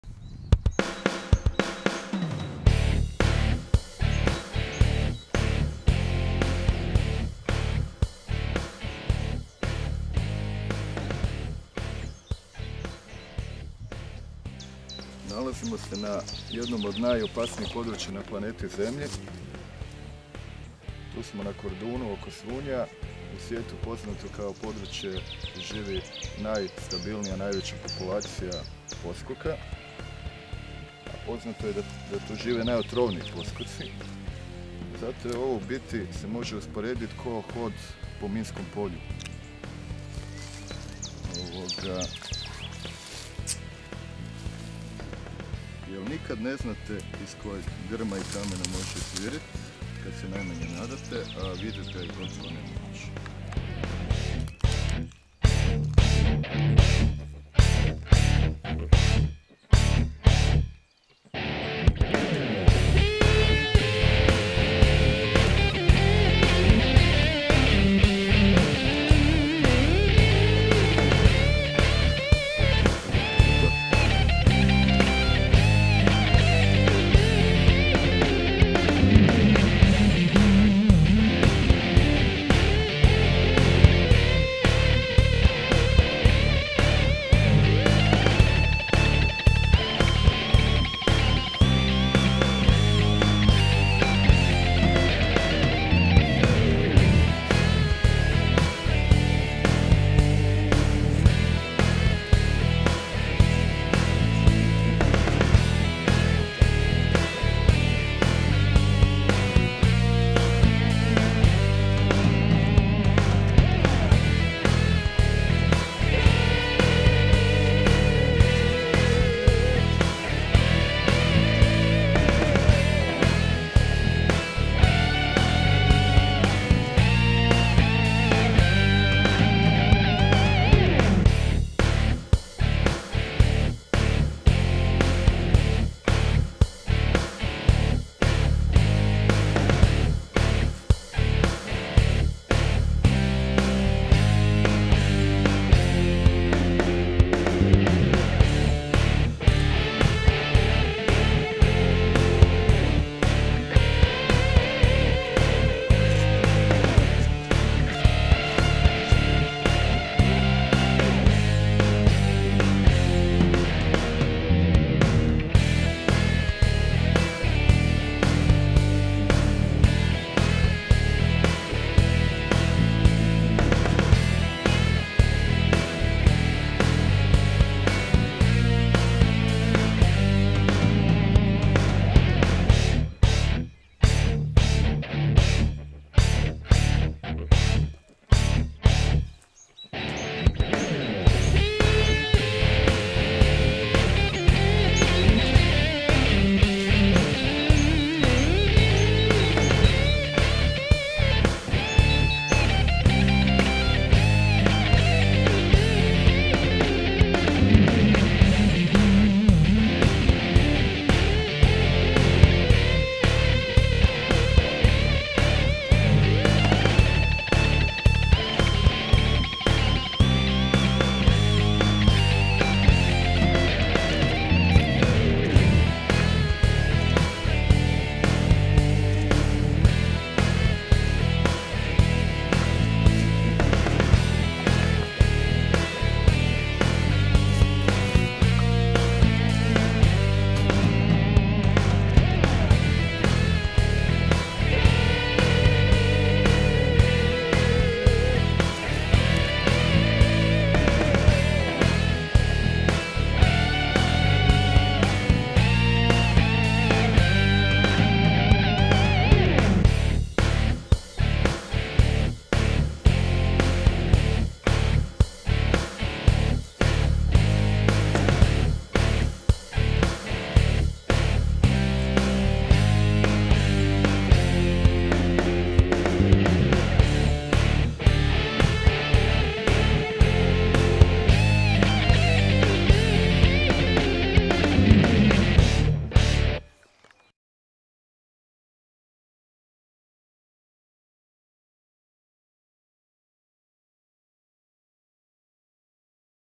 Sutra - "Lovac na krokodile"
Kordunskim kamenjarom u potrazi za otrovnim zmijama - poskocima.